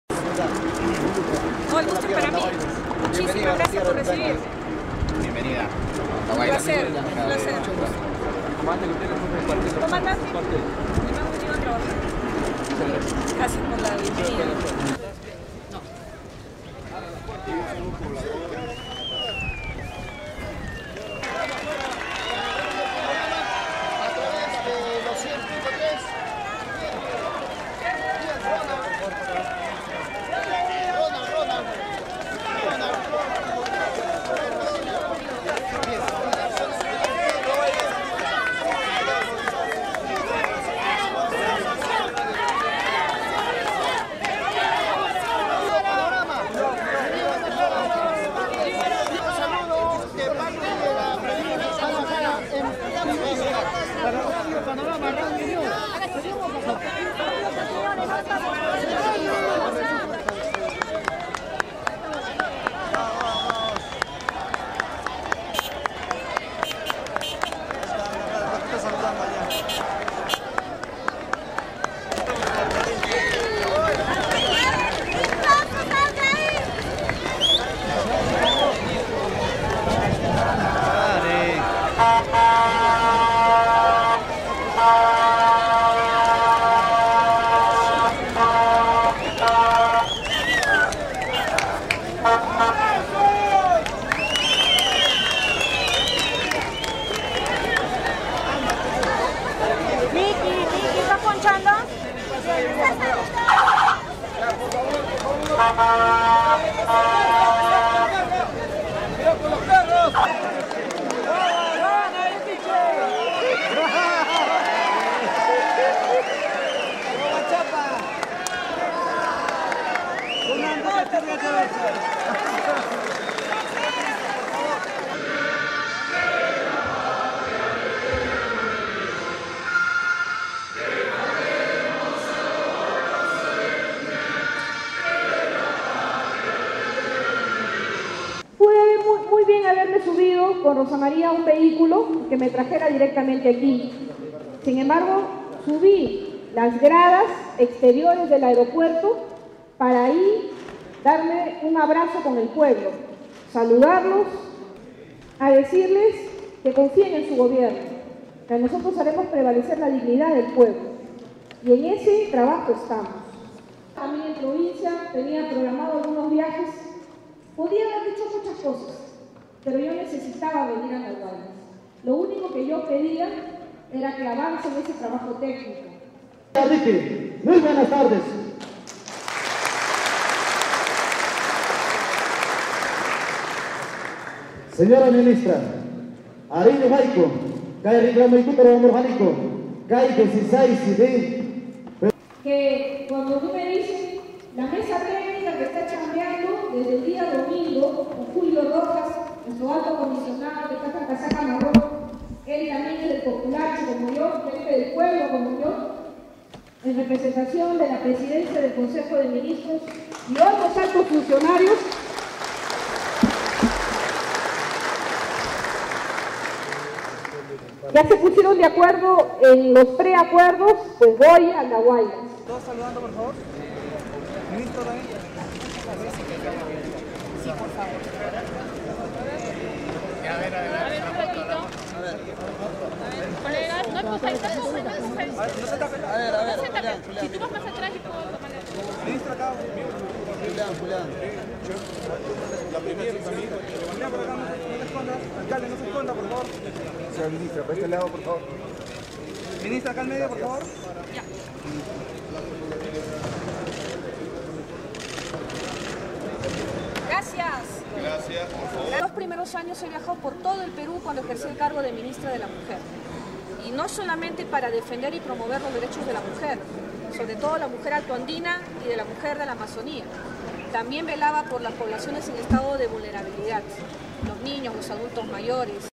Presidenta del Consejo de Ministros se hace presente en Andahuaylas
Presidenta del Consejo de Ministros Ana Jara se hace presente en Andahuaylas para la firma de Acuerdo
Andahuaylas 17 de marzo de 2015

Agrega un comentario